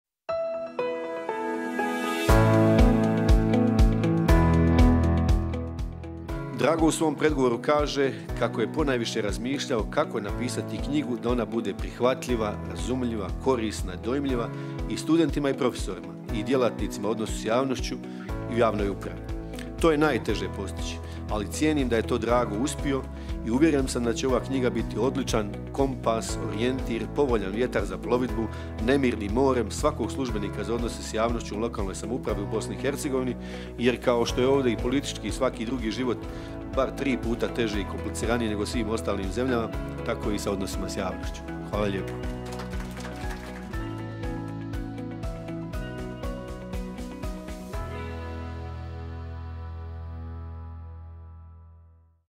Miroslav Landeka o knjizi „Odnosi s javnošću u jedinicama lokalne samouprave“
Miroslav Landeka savjetnik za odnose s javnošću u Gradskoj upravi Grada Mostara jedan je od predstavljača knjige „Odnosi s javnošću u jedinicama lokalne samouprave“ autora doc. dr. sc. Drage Martinovića. Predstavljanje knjige održano je u Širokom Brijegu 1. srpnja 2023. godine.